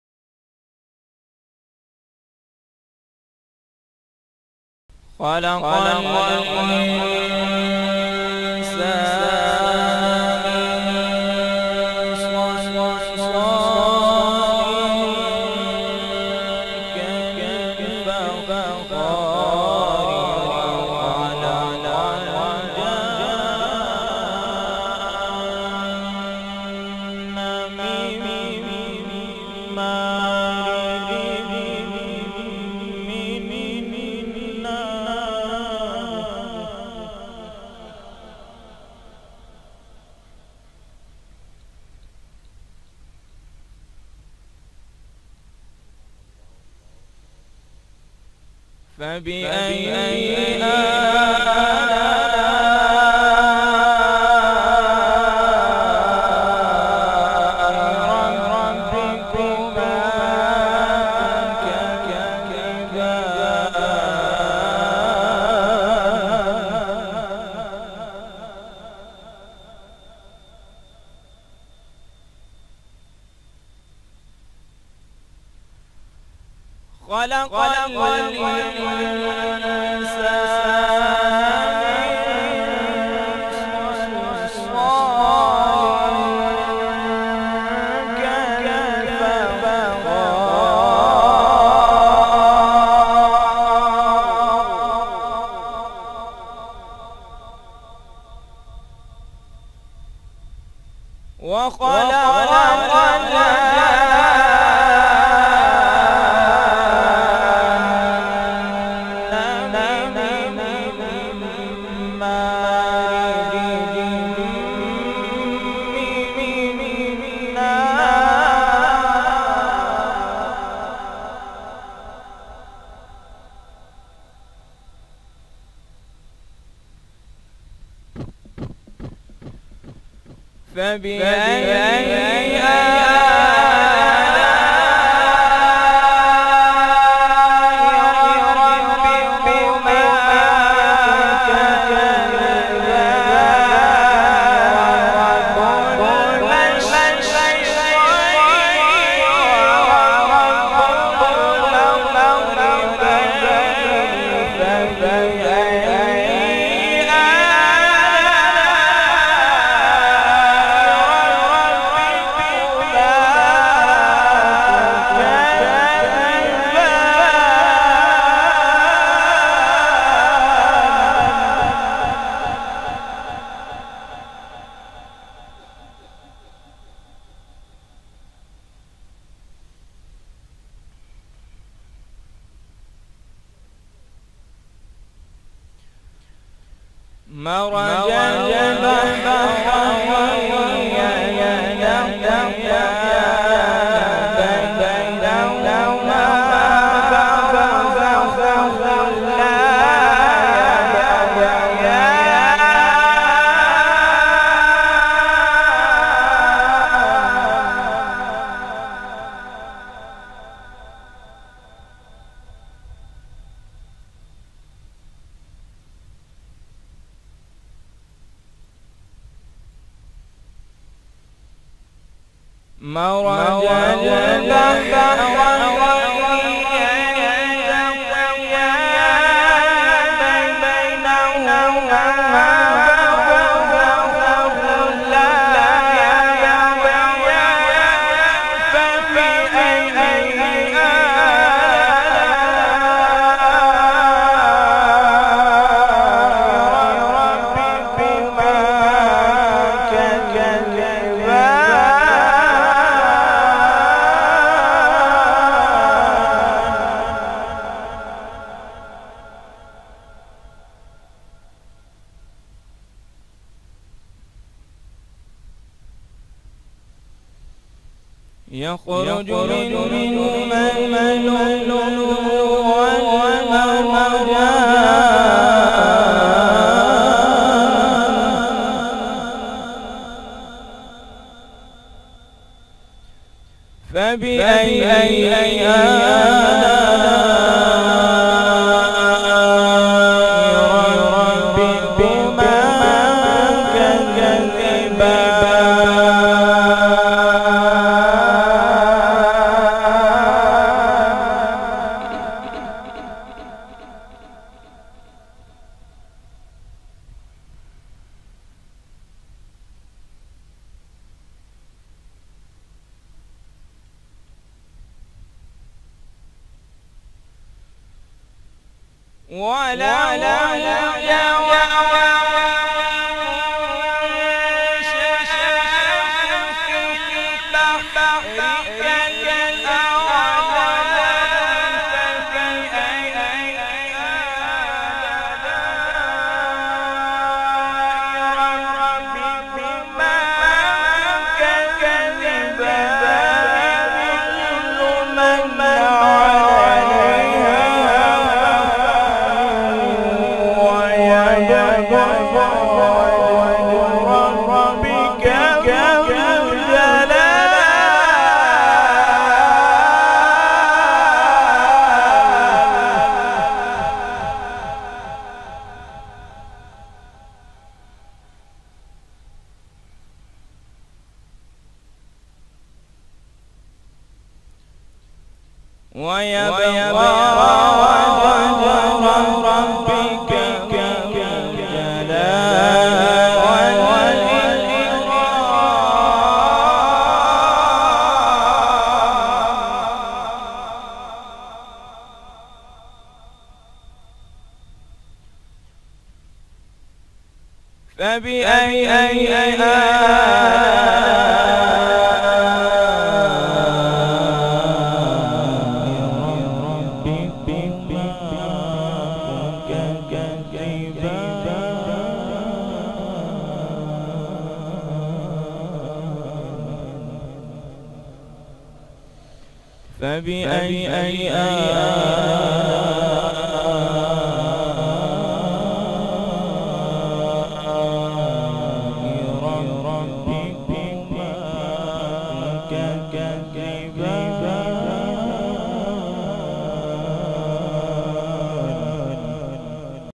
Heart Touching Tilawat Quran by Syed Zeeshan Haider - Beautiful Recitation
Urs Mubarak in Astana e Aliya Gorsian Sharif Nazim-e-Aala Allama Pir Syed Sabir Hussain Shah Qadri Dewna Mendi Gujrat 1 October.
Haq Production Hussain Plaza Fawara Chowk Gujrat Mobile No, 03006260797.